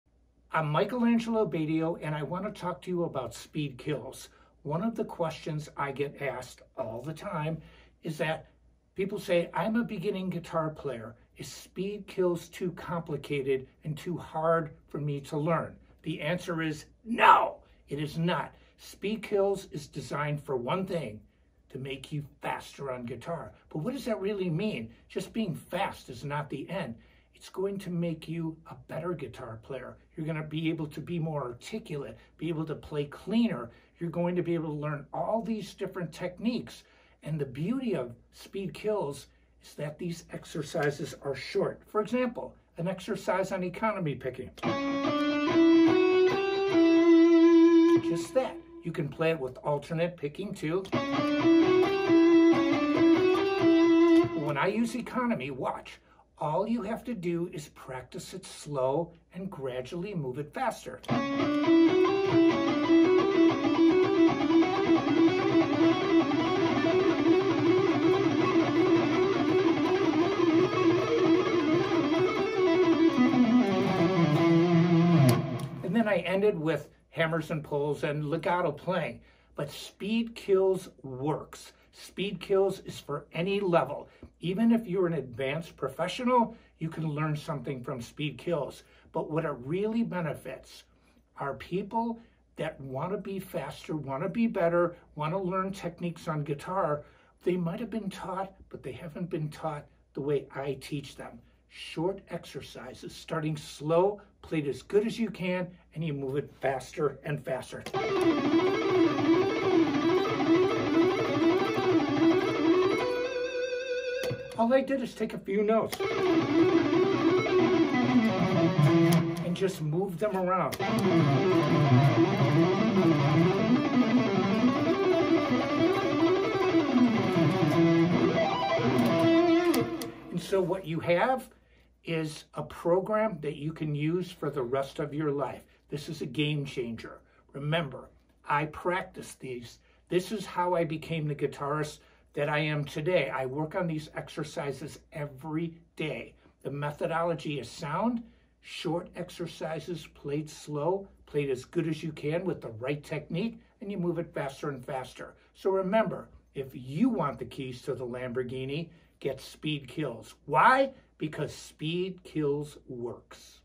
Speed Kills by Michael Angelo Batio - Not Just for Shredders
Michael explains why his legendary program is for all levels of playing experience. In fact, those that will get the most out of the program are intermediate players. Don't let fear hold you back from reaching your potential. This is the program that today's top players have been studying since 1991.